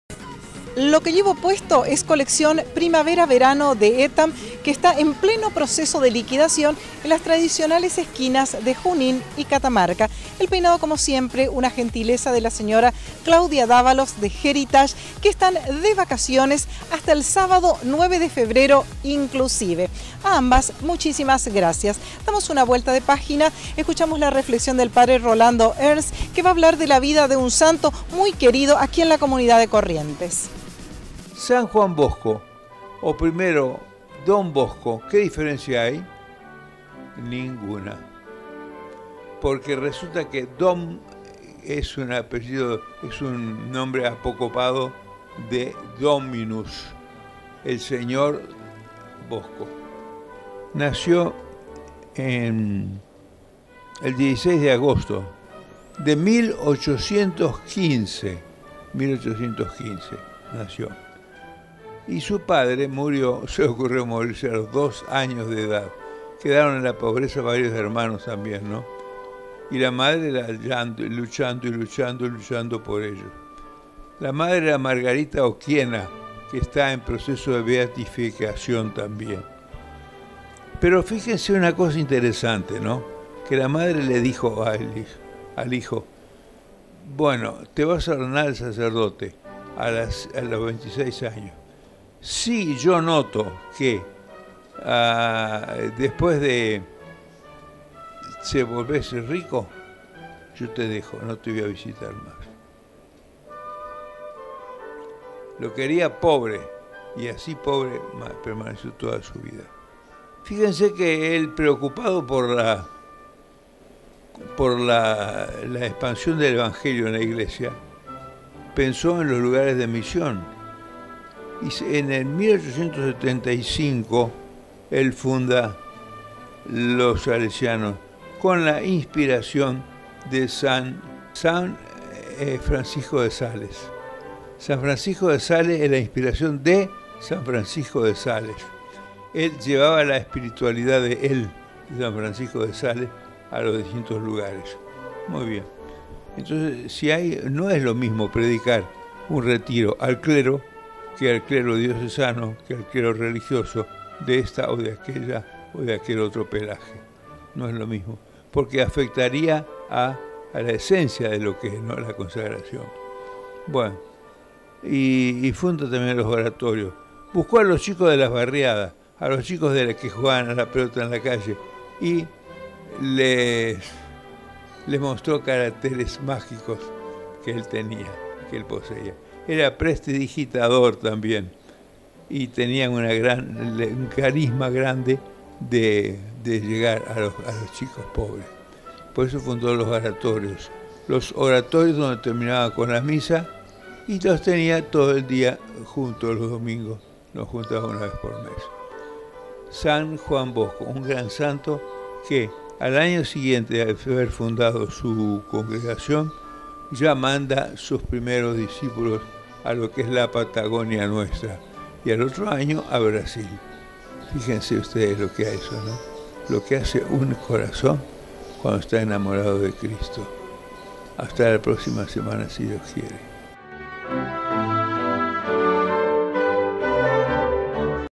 Padre Rolando Ernst
Hoy el Padre Rolando nos recuerda a DON BOSCO . . .